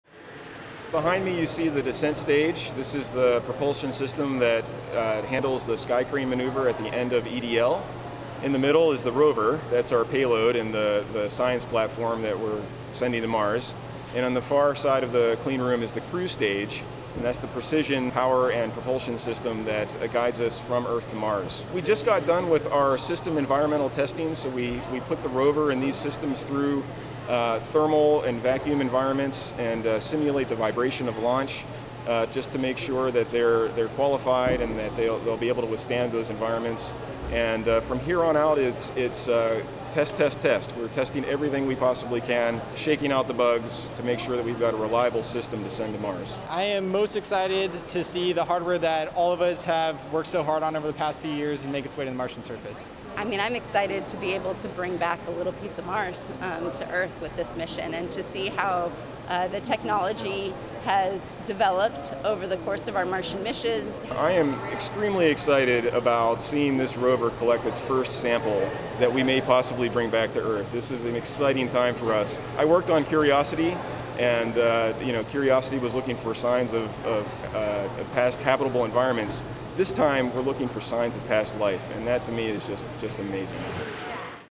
NASA shows off Mars 2020 rover progress
NASA is giving people an up-close look at the new Mars rover. Members of the media were invited into the cleanroom at NASA's Jet Propulsion Laboratory Friday.